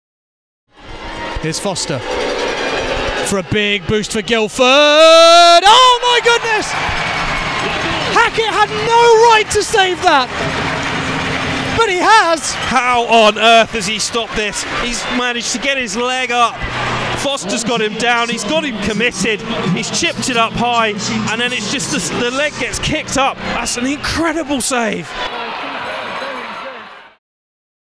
Hackett's Shoot Out Save - 13/01/19
Matt Hackett pulls off an incredible save in the shoot out